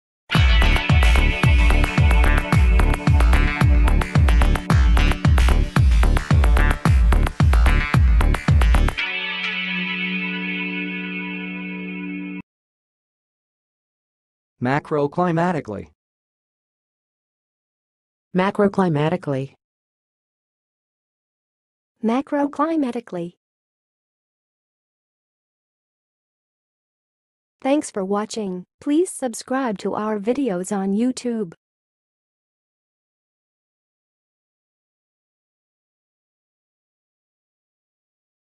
How to say "macroclimatically"! (High Quality Voices)
Watch in this video how to say and pronounce "macroclimatically"!